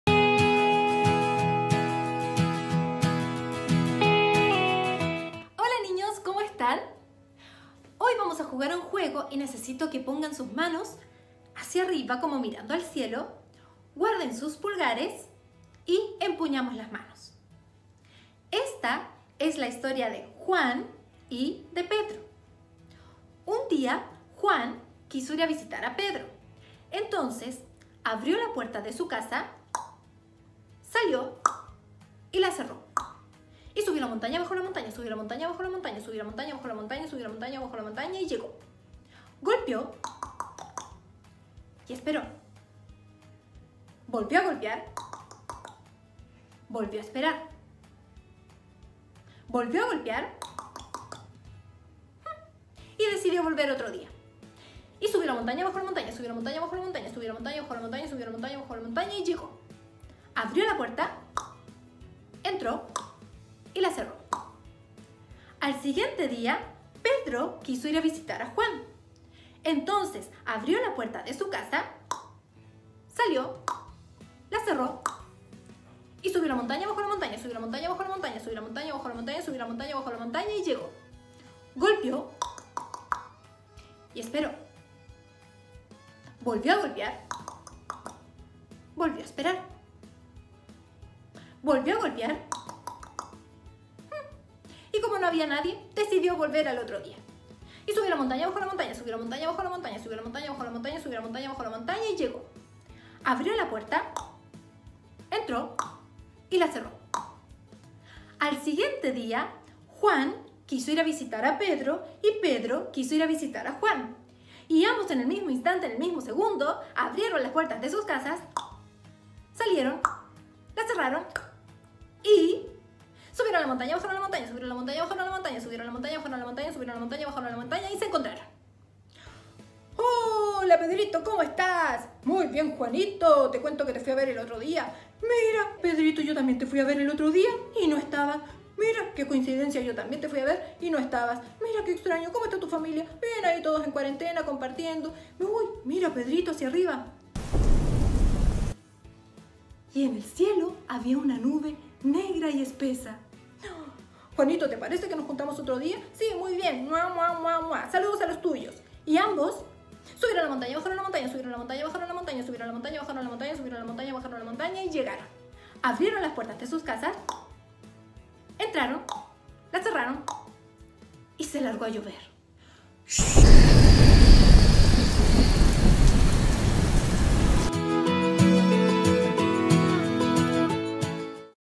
Historia de Juan y Pedro
Comprensión Lectora: Comunicación Integral / Lenguaje Verbal
Objetivo de Aprendizaje: 6. Comprender contenidos explícitos de textos literarios y no literarios, a partir de la escucha atenta, describiendo información y realizando progresivamente inferencias y predicciones.